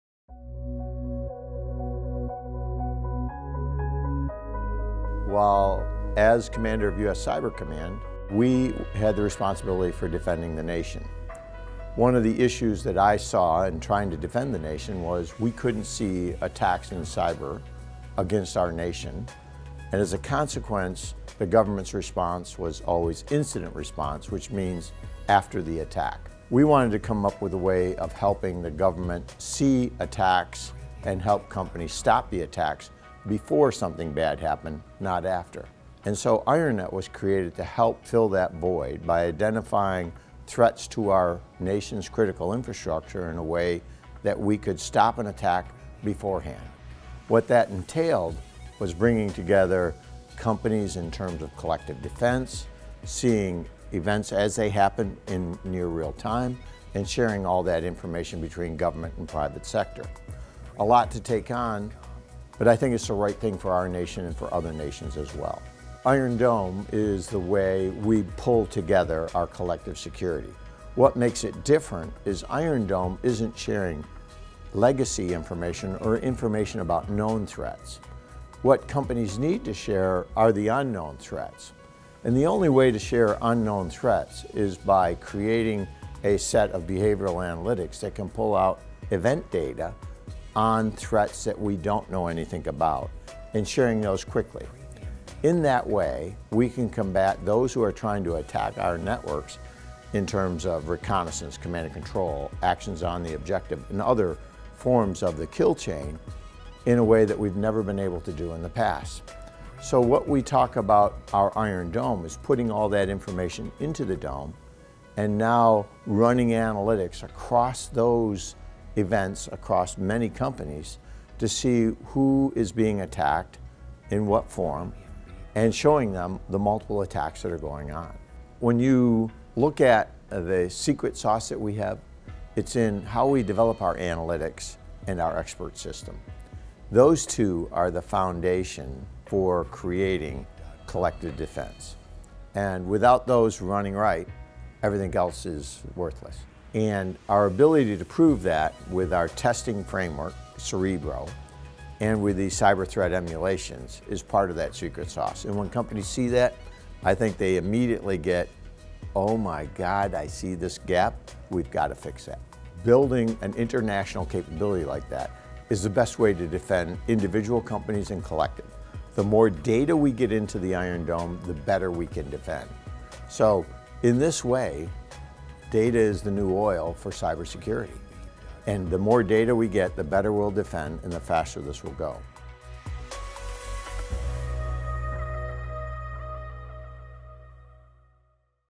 Collective Defense is the future of cybersecurity
IronNet co-Founder GEN (Ret.) Keith Alexander discusses the cyber challenges enterprises face today and why he founded IronNet to solve these problems.